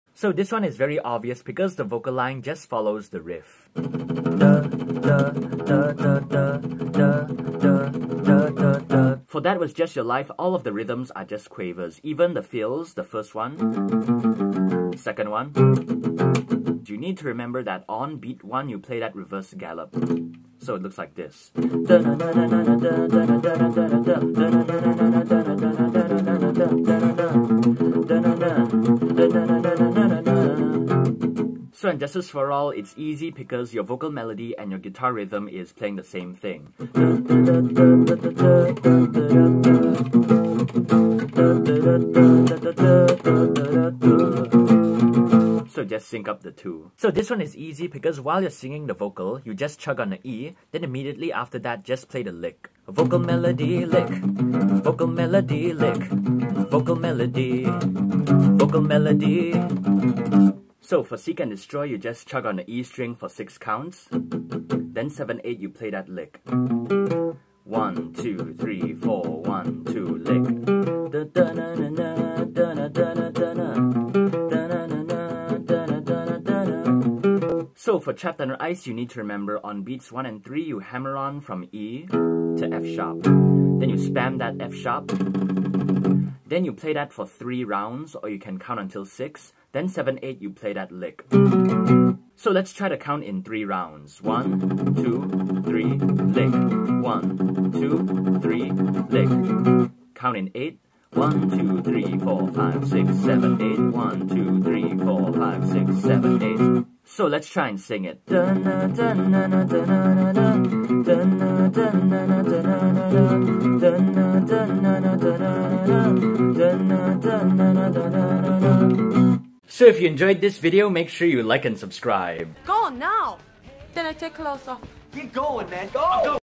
easy Metallica songs to sing & play ( at the same time )
what are the easiest Metallica songs to sing and play guitar at the same time?

0:00 Song 1
0:09 Song 2
0:31 Song 3
0:47 Song 4
1:00 Song 5
1:17 Song 6